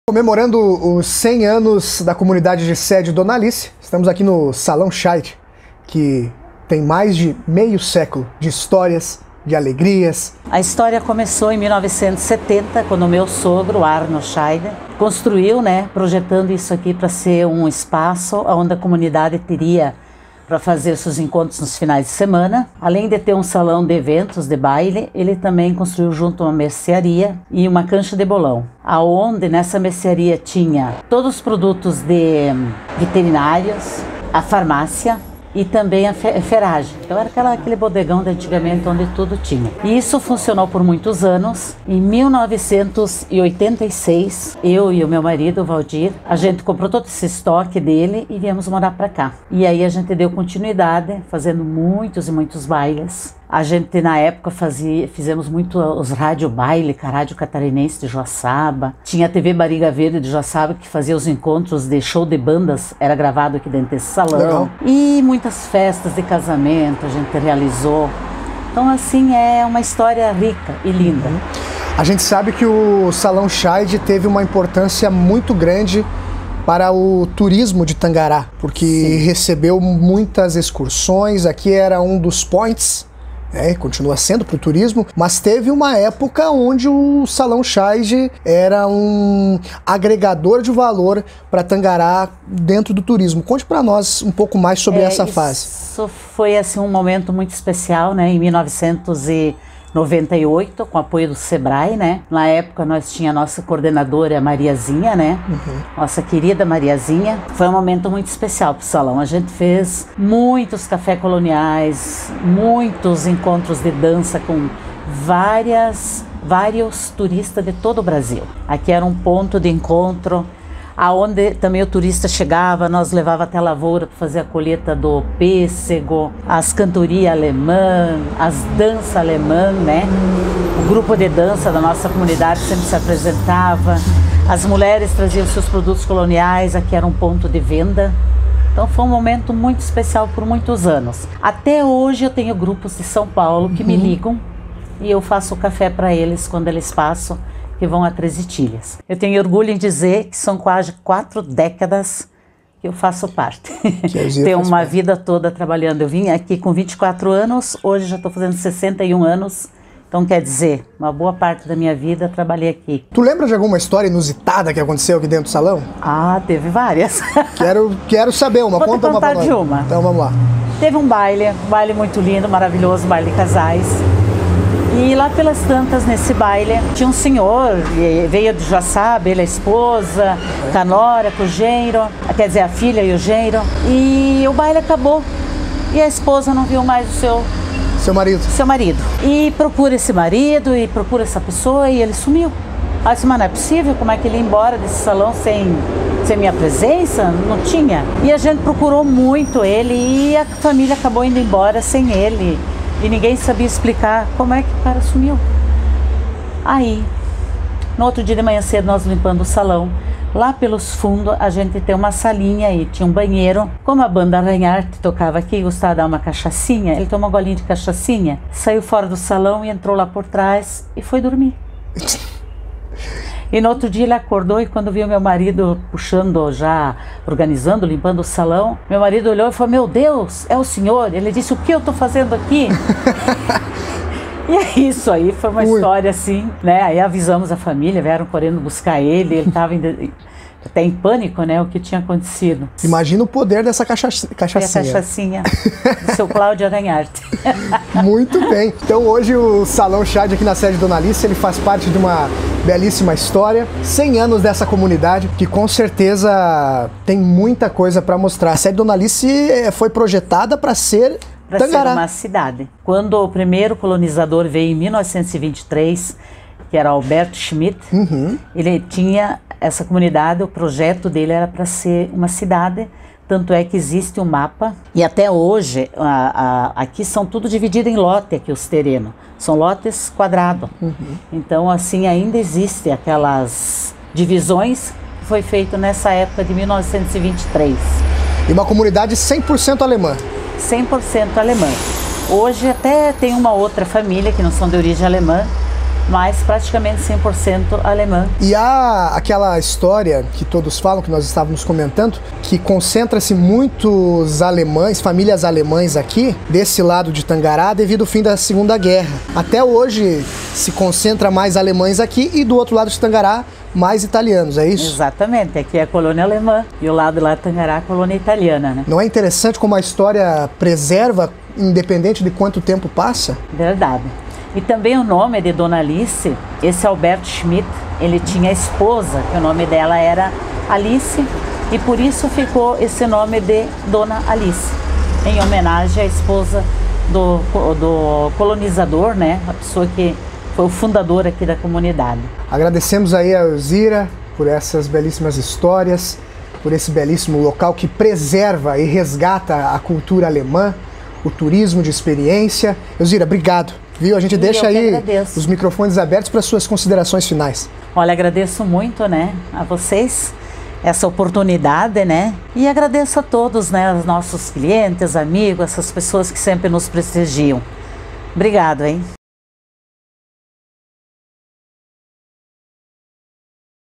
Clube no interior de Tangará há mais de meio século vem preservando a cultura alemã
Comemorando os 100 anos da comunidade de sede dona Alice em Tangará, vamos conhecer um pouco mais sobre o Salão Sheid, com mais de meio século de vida, preservando a cultura alemã e sua estrutura até os dias de hoje.